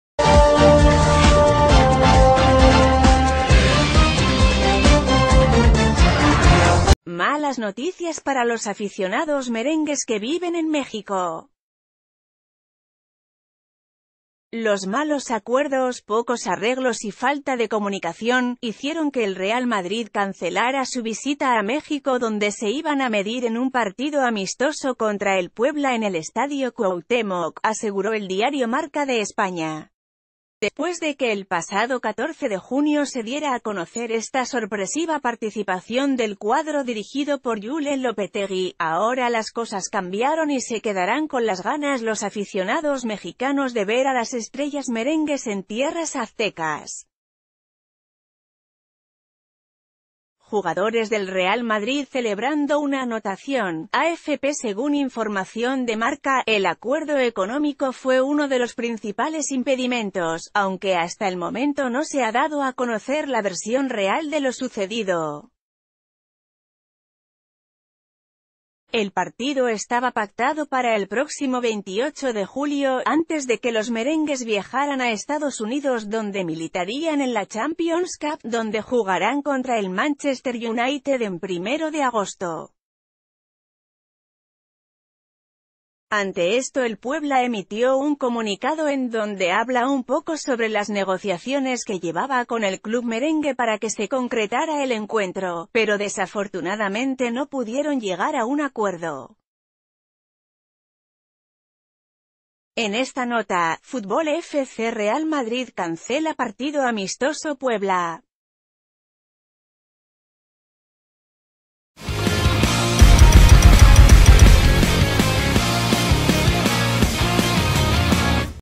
Se cancela el partido amistoso del Real Madrid ante Puebla
Malas noticias para los aficionados merengues que viven en México. Los malos acuerdos, pocos arreglos y falta de comunicación, hicieron que el Real Madrid cancelara su visita a México donde se iban a medir en un partido amistoso contra el Puebla en el Estadio Cuauhtémoc, aseguró el diario Marca de E...